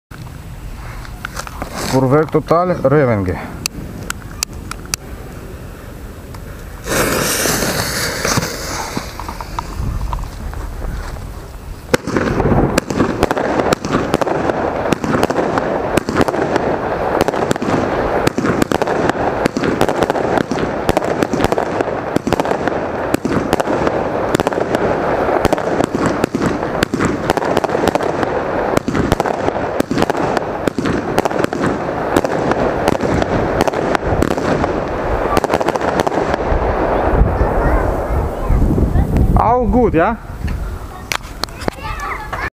Vuurwerktotaal Revenge
Silvesterfeuerwerk 2017/18
Effekt ist nicht so wie Muster, aber was für fetten Blinker.